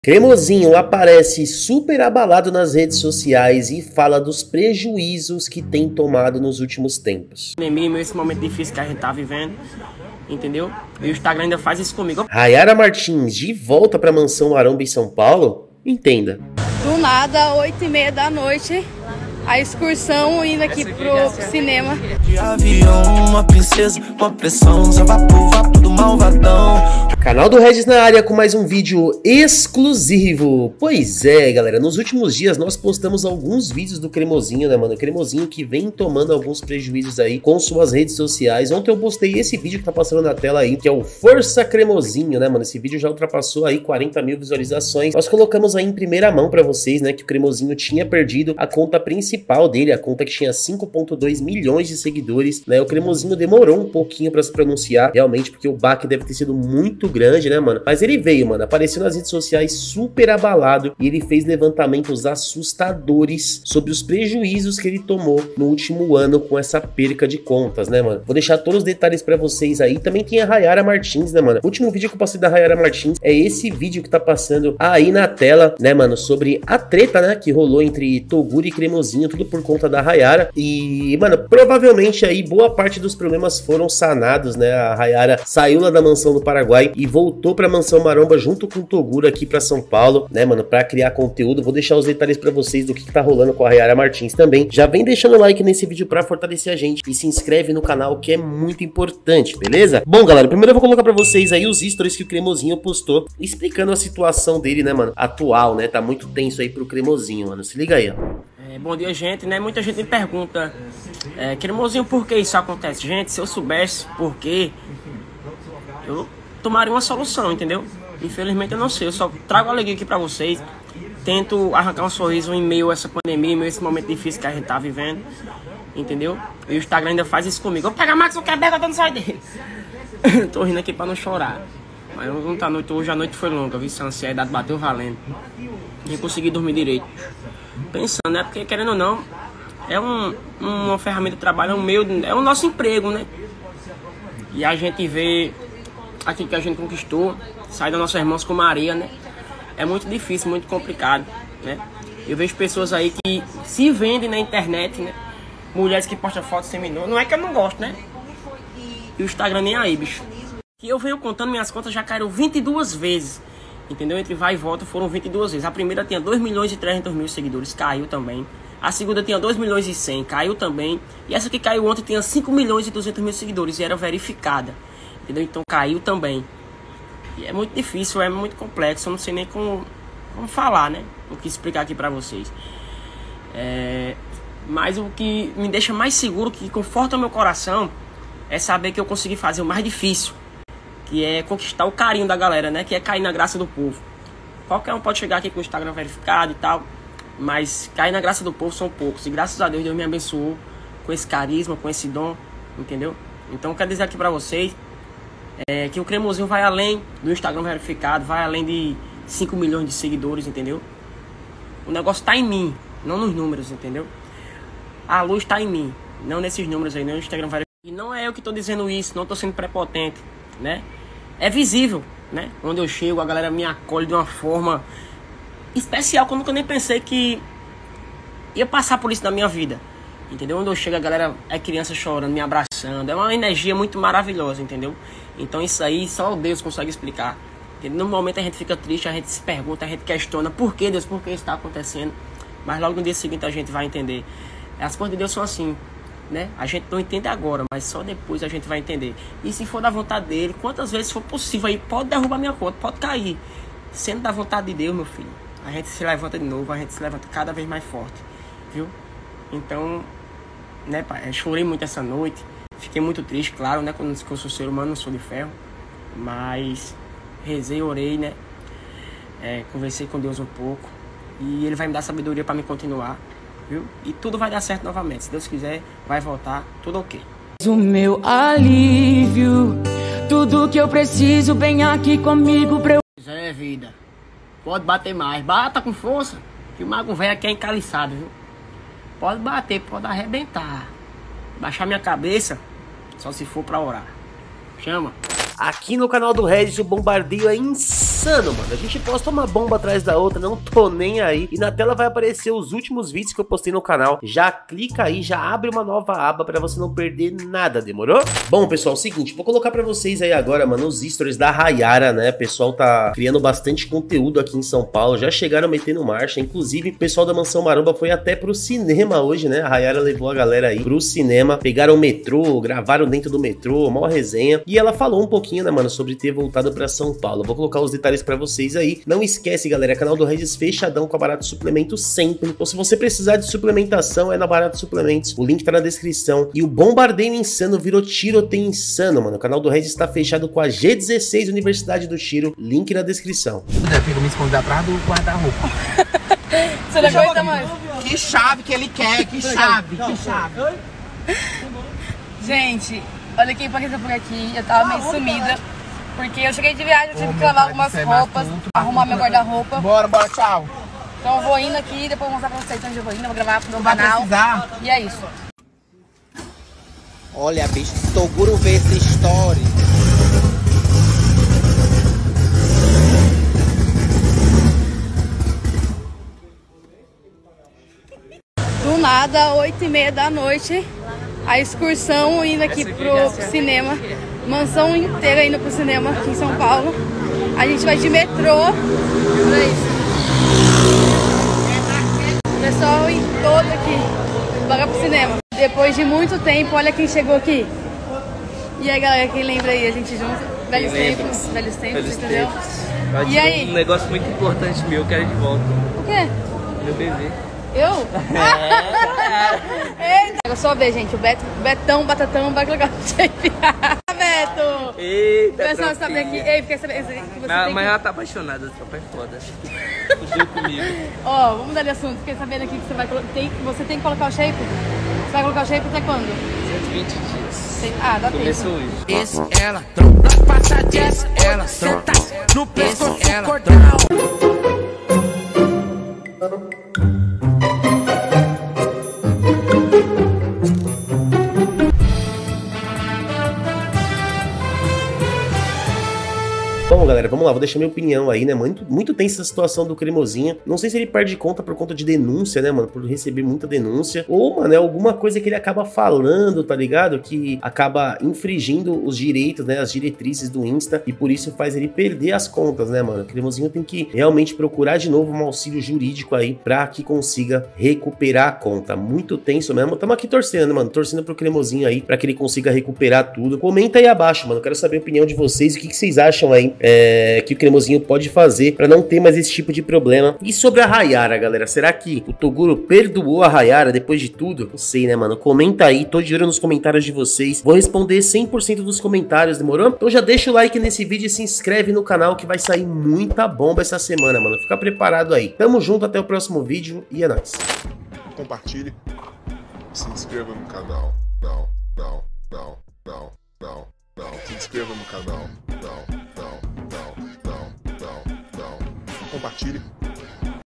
CREMOSINHO ESTÁ SOFRENDO NA NOSSA FRENTE! RAIARA VOLTOU PARA MANSÃO MAROMBA SP!
🔥Você encontra suplementos de todas as marcas com o

G16 Universidade do Tiro: o clube 24h mais exclusivo do Brasil.

Opa galera Canal do Regis na área com mais um vídeo exclusivo
Pois é galera

00:00 Inicio
00:27 Assunto do Vídeo
01:48 Stories do Cremosinho
04:55 Bombardeio Insano
05:08 Raiara voltou pra mansão maromba?
07:22 Opinião do Regis e Fim